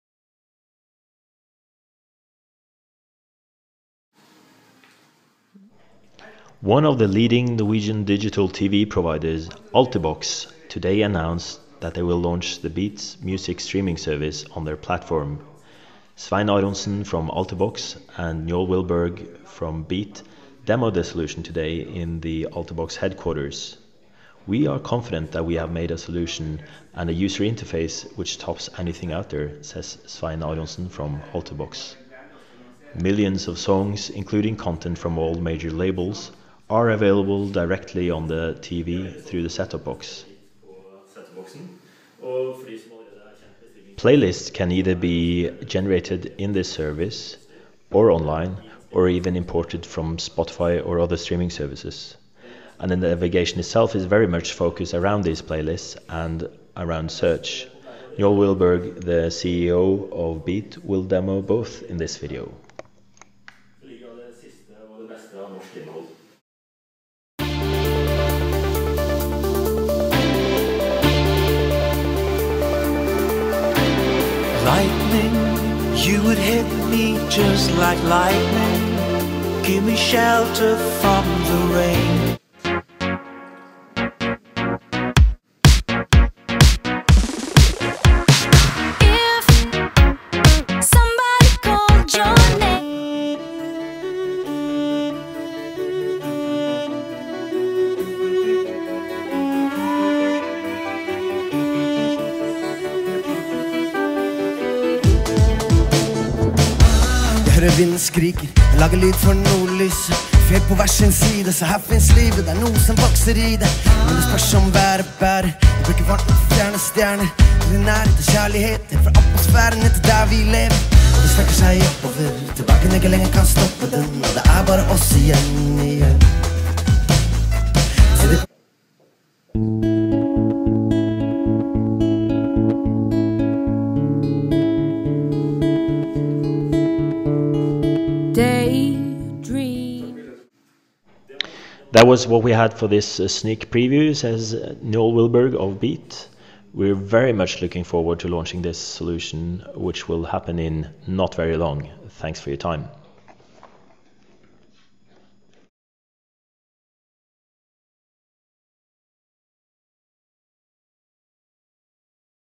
Altibox and Beat launches digital TV streaming service
Altibox and Beat announces the launch of Music Streaming Service in the Altibox Platform.

Altibox is one of the leading Digital TV providers in Norway.

Beat.no is a music service startup from Mjømna.